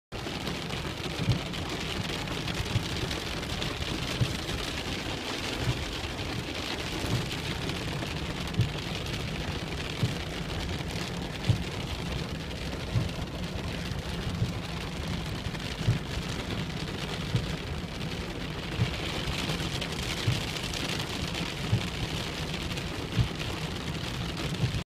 Raining hard on the westside right now.
By: Carlos Illescas - Denver Post reporter covering Aurora and South Metro Denver. Also fill in as editor.